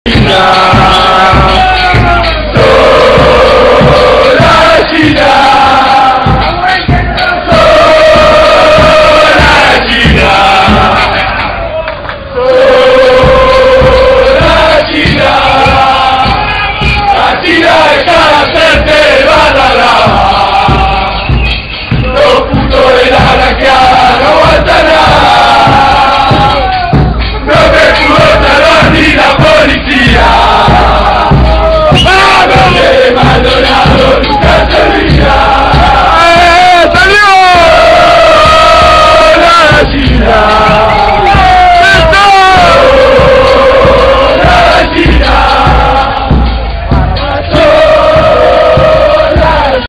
Hinchada de Peñarol en el Obelisco | Copa Libertadores 2011
La hinchad de Peñarol en el Obelisco de la ciudad de Buenos Aires, Argentina, en la previa al partido contra Independiente por la 1era. Fecha de la Copa Libertadores de América 2011.